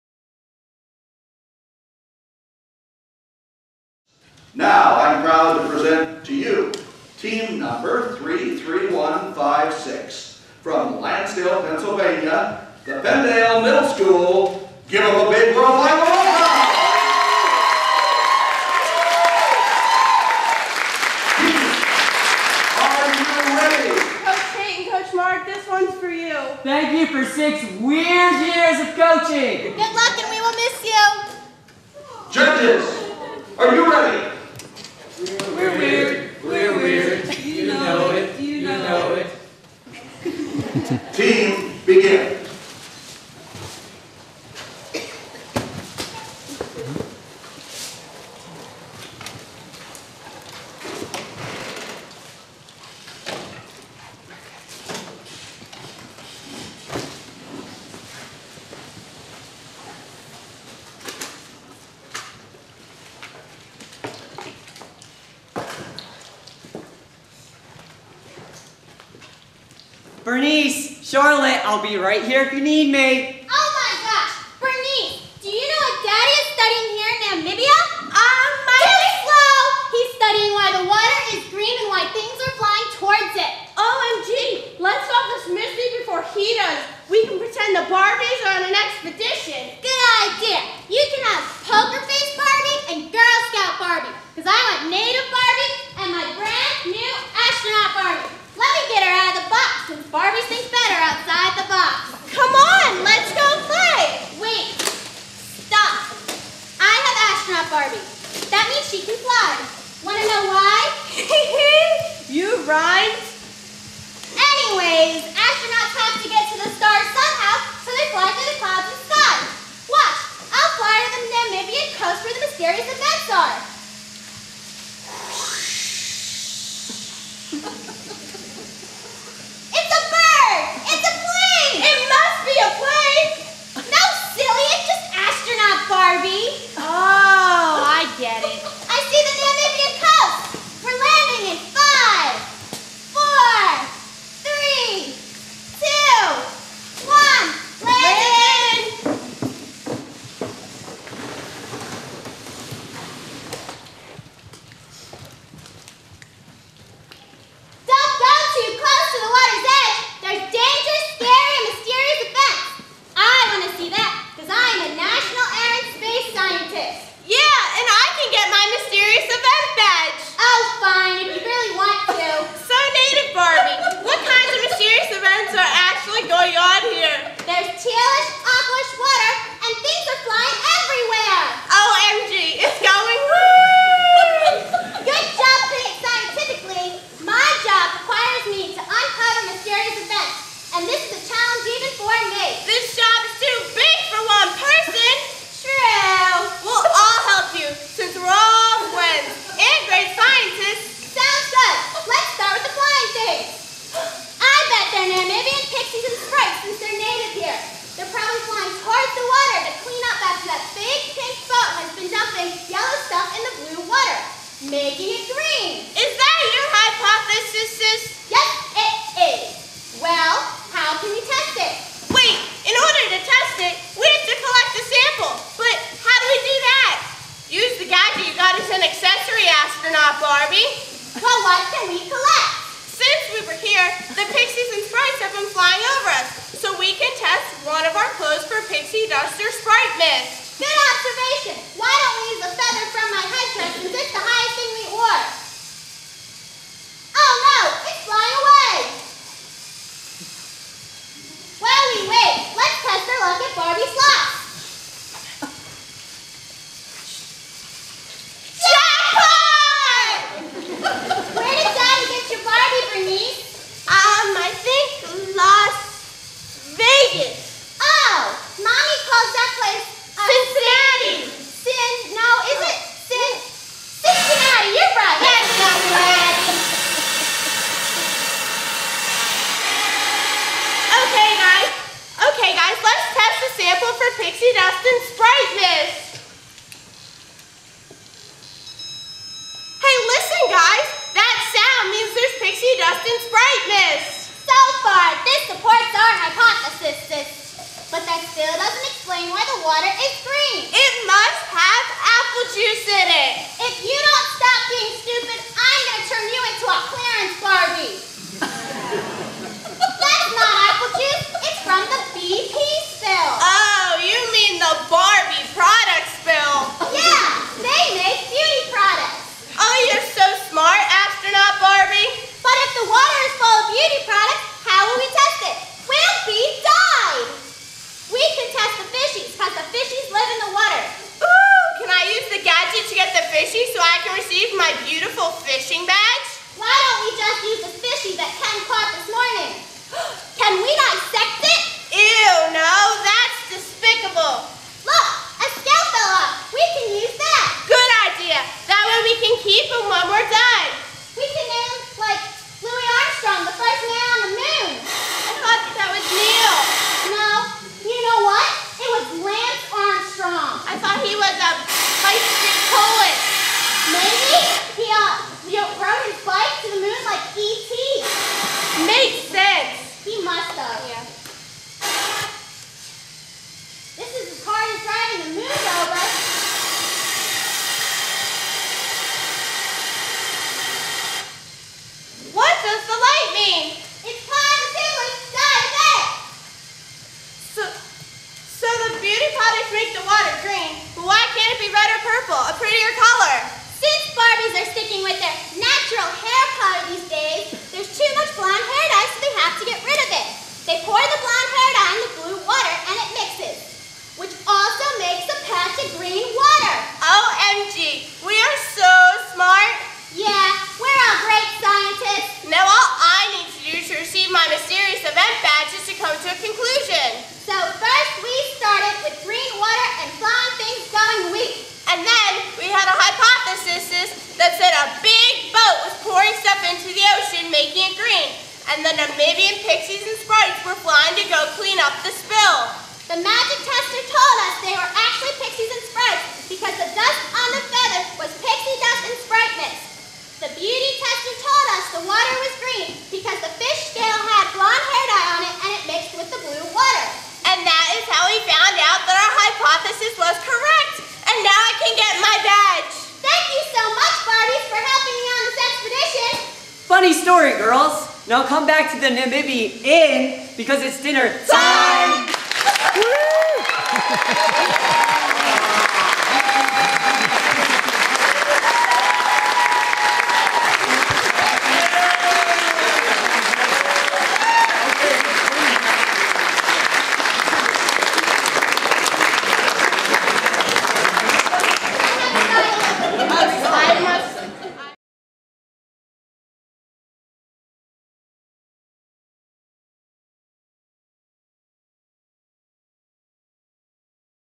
2012 Odyssey of the Mind World Finals - Penndale Middle Sch - Weird Science Div 3
This is our team's performance from the 2012 World Finals competition in Ames, Iowa.
Team Name: Penndale MS (From Lansdale, PA)
Division 3
Problem 2 - Weird Science